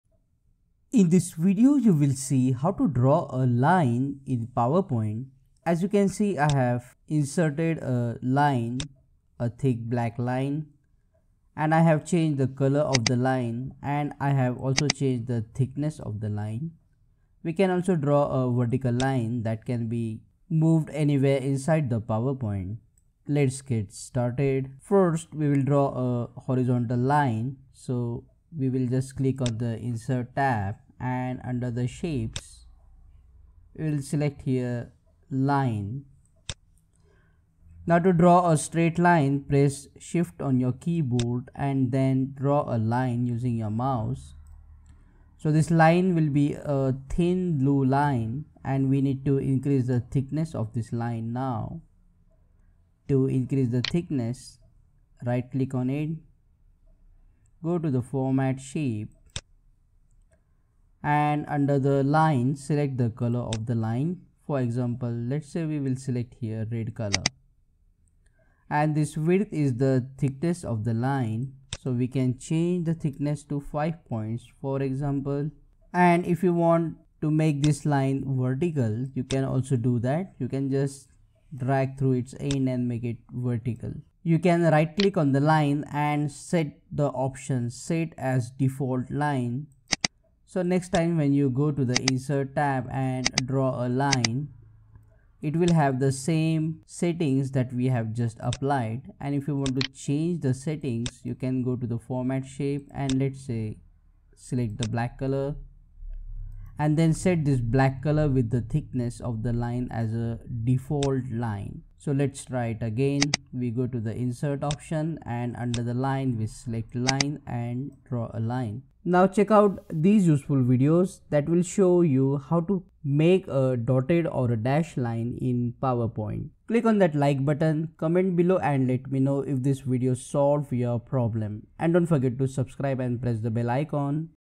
How to Draw a Line in PowerPoint (PPT)
Watch How to draw a Line in Powerpoint (PPT) slide in this video and make the line a thick line. To draw a straight line, horizontal or vertical press SHIFT on the keyboard then draw a line to get the straight line in Powerpoint